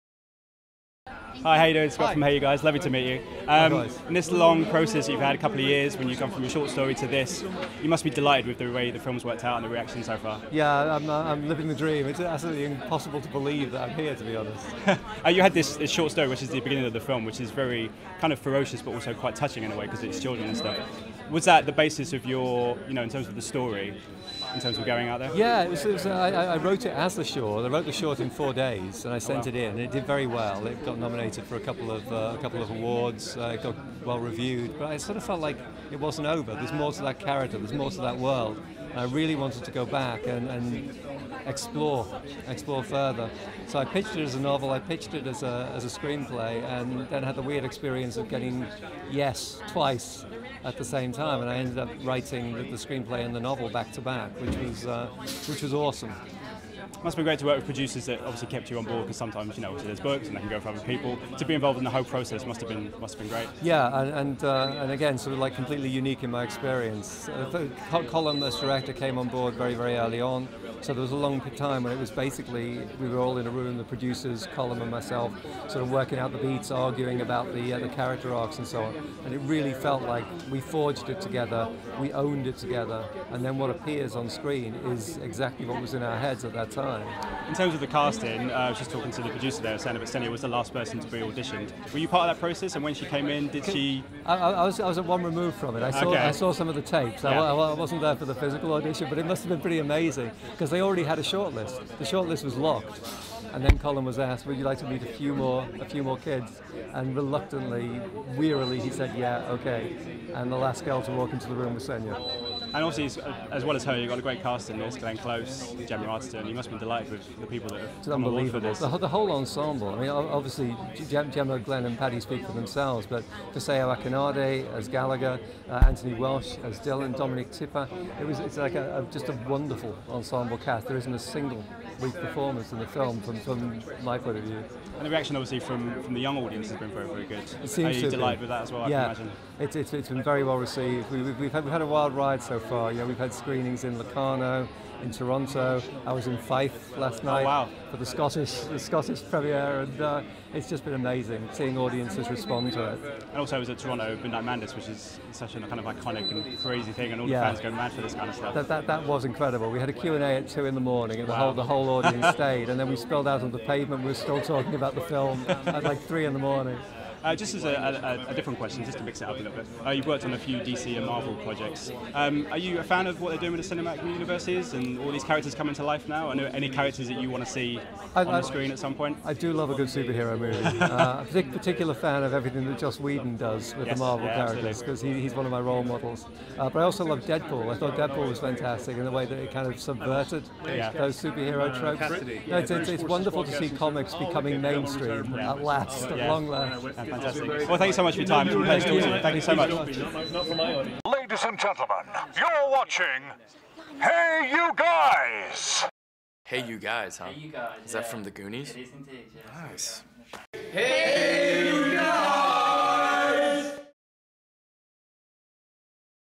Writer Mike Carey Premiere Interview - The Girl with All the Gifts
Our interview with Writer Mike Carey for the sci-fi horror  The Girl with All The Gifts.

The film stars  Gemma Arterton, Paddy Considine, Glenn Close, Sennia Nanua and is directed by Peaky Blinders' man Director
Colm McCarthy

Plot:
The near future; humanity has been all but destroyed by a mutated fungal disease that eradicates free will and turns its victims into flesh-eating “hungries”. Only a small group of children seem immune to its effects. At an army base in rural England, this group of unique children are being studied, subjected to cruel experiments by biologist Dr. Caldwell. The children attend school lessons daily, guarded by the ever watchful Sergeant Parks. But one little girl, Melanie, stands out from the rest. She excels in the classroom, is inquisitive, imaginative and loves her favourite teacher Miss Justineau. When the base falls, Melanie escapes along with Miss Justineau, Sergeant Parks and Dr. Caldwell. Against the backdrop of a blighted Britain, Melanie must discover what she is and ultimately decide both her own future and that of the human race.